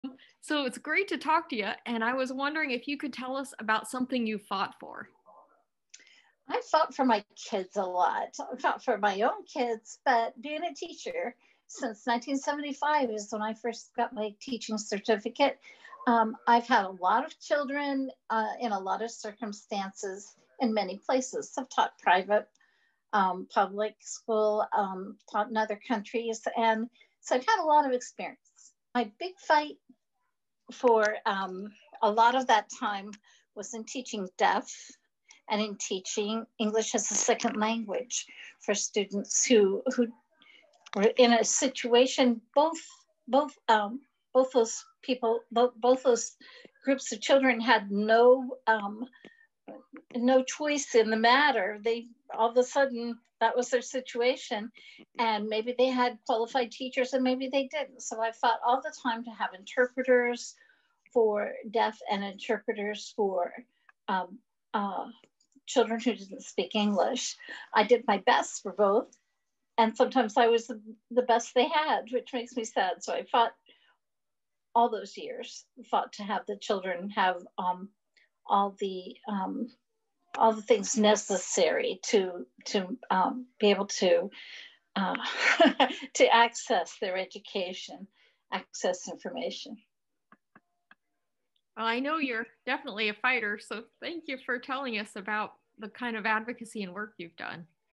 Wizards and Weirdos Item#5
Livestream Challenge: (UP TO 2 MINUTES) In his panel, Jay Jasper Pugao spoke about how many of the books and written history of the Filipino people were burned by Spanish conquerors, leaving the oral stories to keep their histories alive. There’s someone in your life whose story is at risk of being lost, too: honor an elder in your life (a person over 65 years of age) and interview them (remotely) about something they fought for that’s close to their heart.